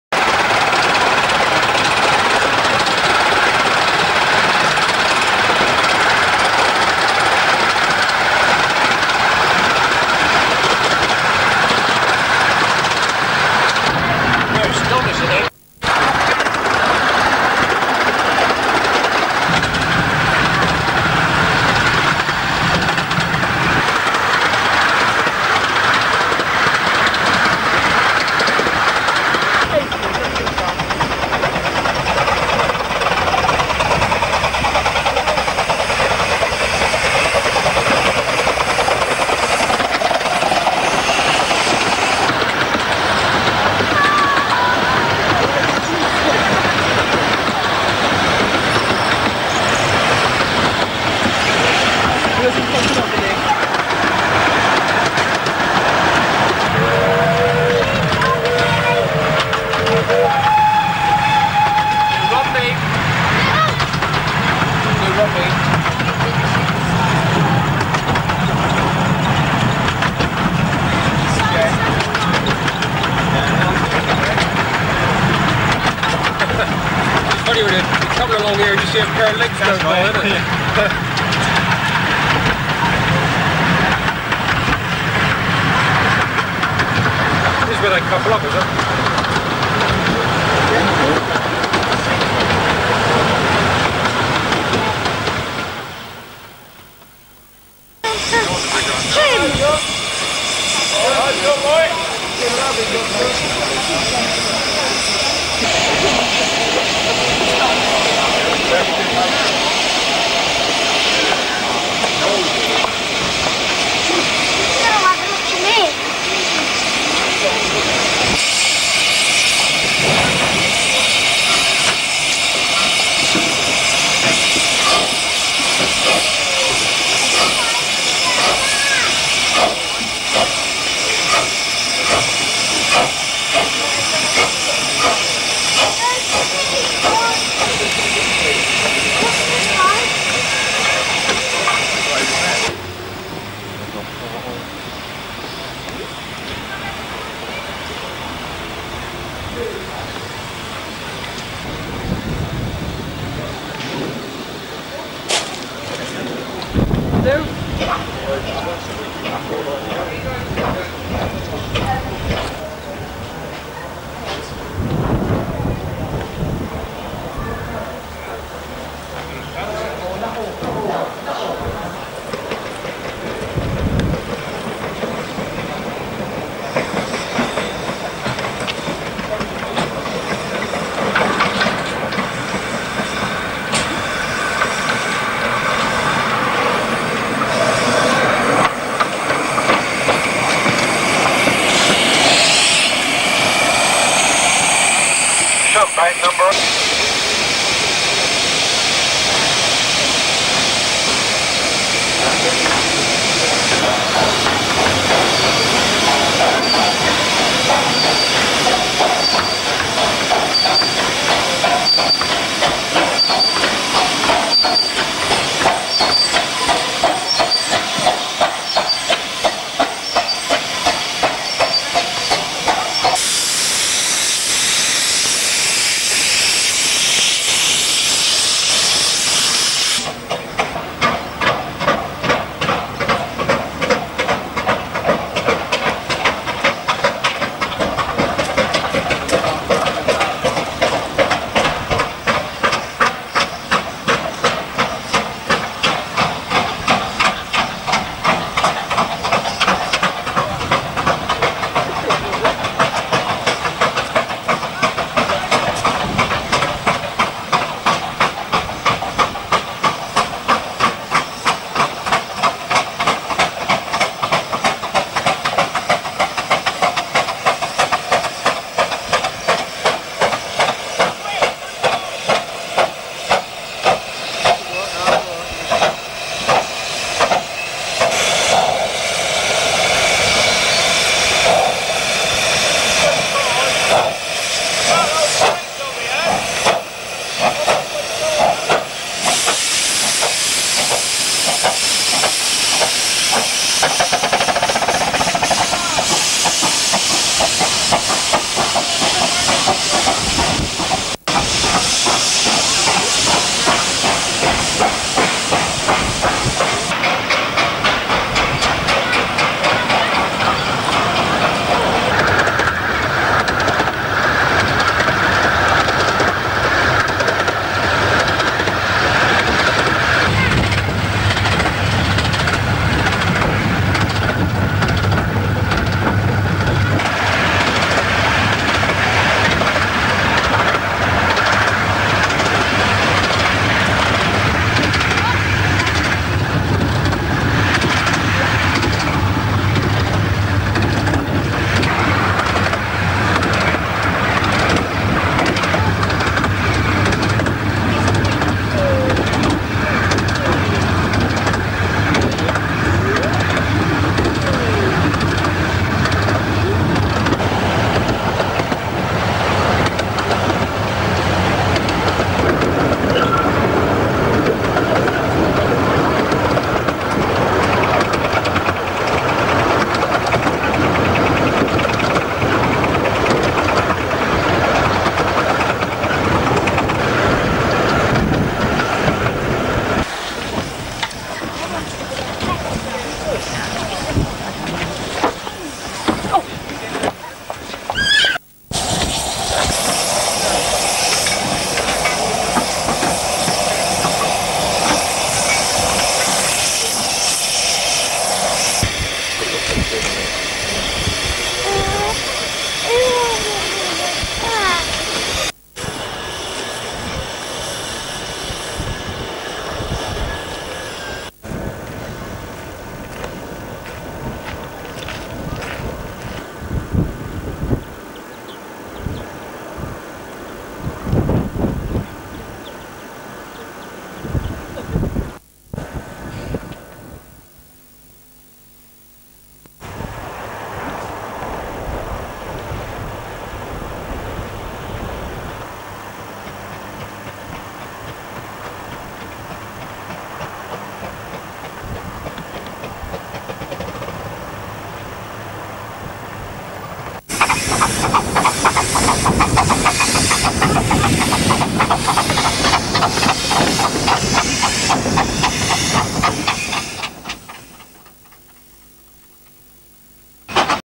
Romney Hythe and Dymchurch Railway "Romney Rally" Part 2.avi
VHS to DVD to divx
Romney Mini Railway Rally Part 2
Taken in the UK 1990
Travel and Events category